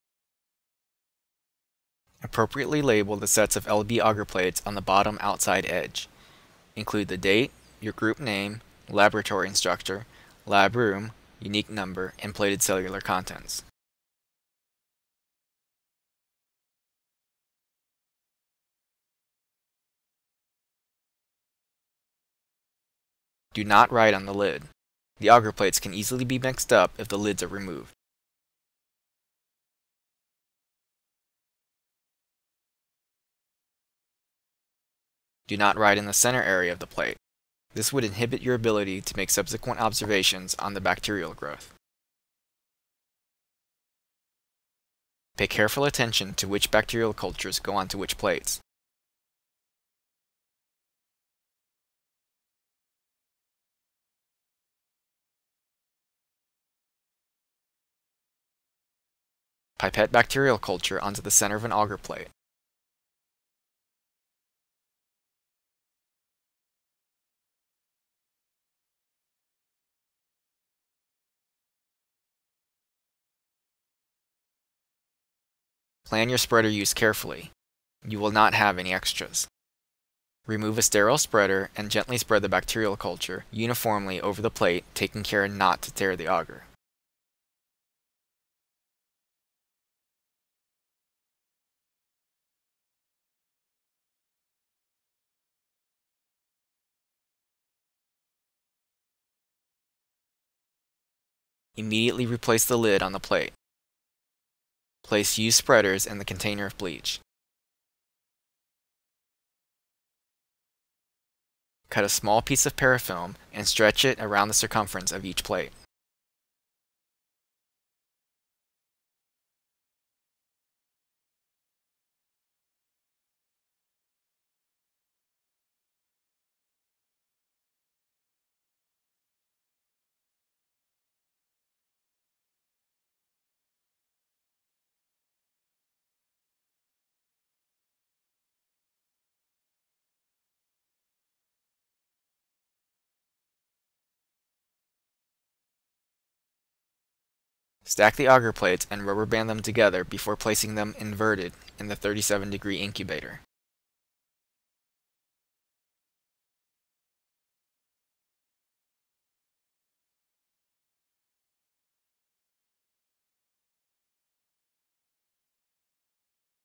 Plating Bacteria Cultures
Spread bacteria cultures on agar plates.